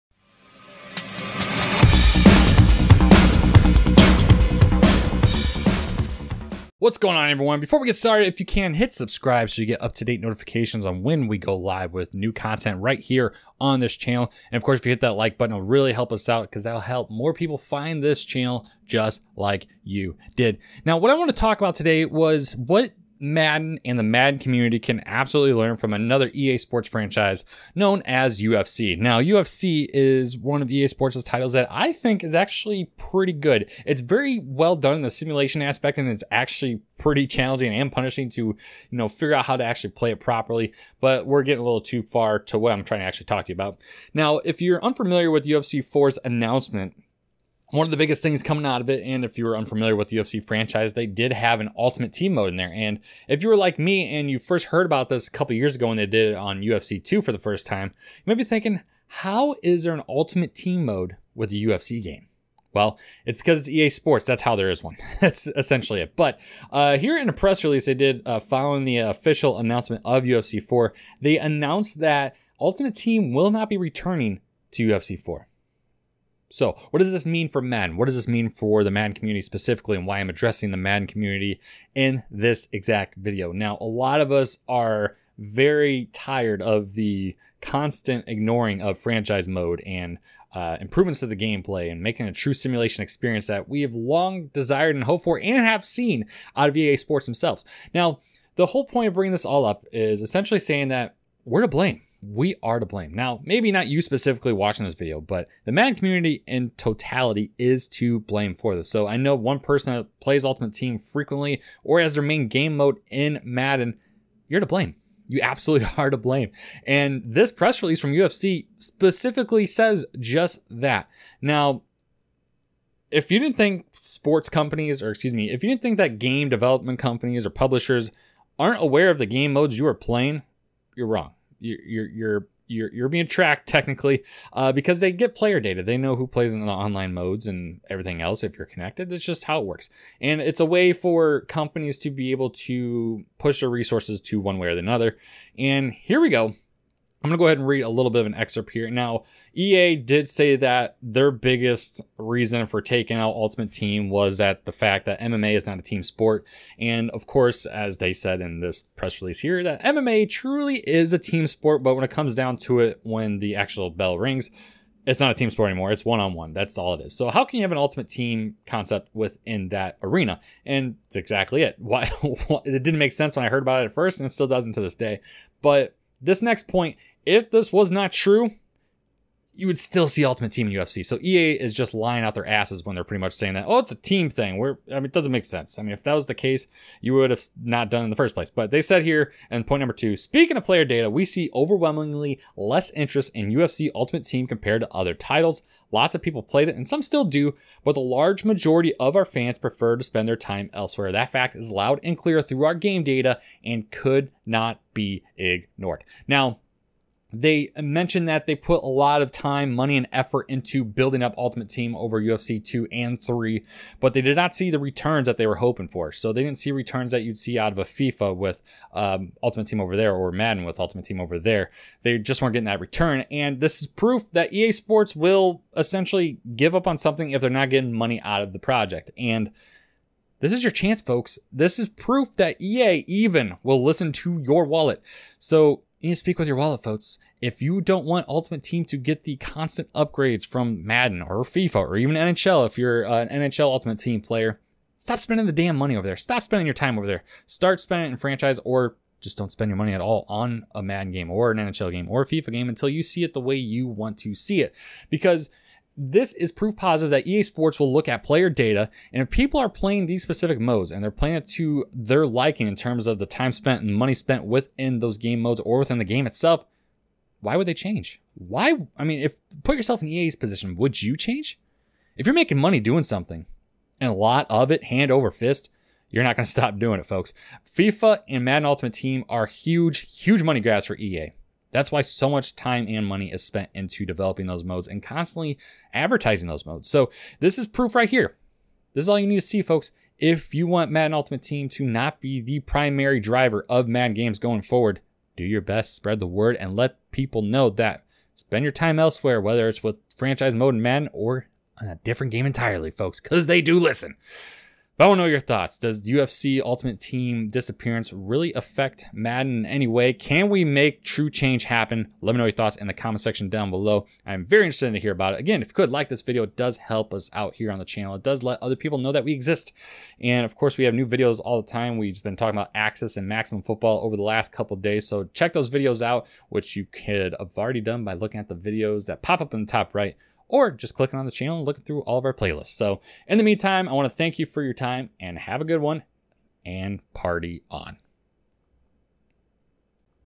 What the Madden Community NEEDS to Learn from the UFC 4 Reveal
UFC 4 was officially announced about a week or so ago - and with it came some unbelievable news from EA Sports and that was the discontinuation of Ultimate Team in UFC games. What can the Madden community learn from this?